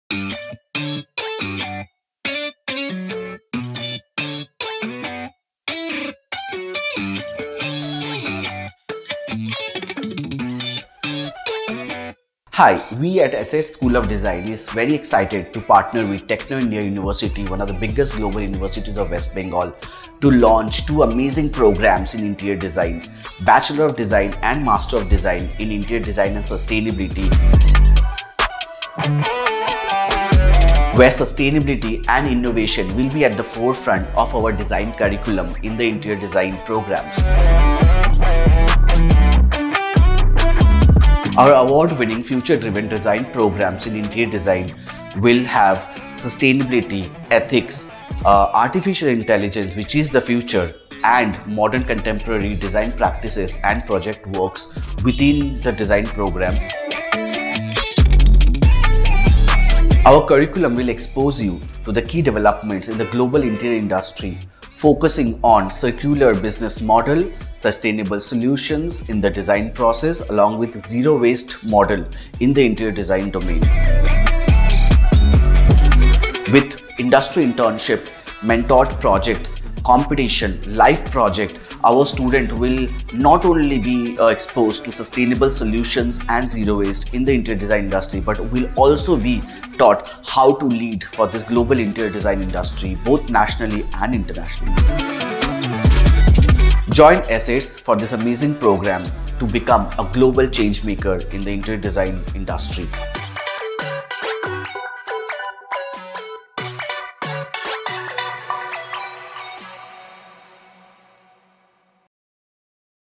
BDes & MDes in Interior Design & Sustainability || ESEDS School of Design
Join the next generation of interior designers leading the charge towards a sustainable future!

At ESEDS School of Design, our MDes and BDes programs in Interior Design and Sustainability will equip you with the skills and knowledge to create spaces that not only inspire, but also reduce environmental impact.

Learn from industry experts and gain hands-on experience in our state-of-the-art facilities. Our programs cover sustainable materials, energy-efficient design, and eco-friendly practices.

Apply now and be part of the design revolution!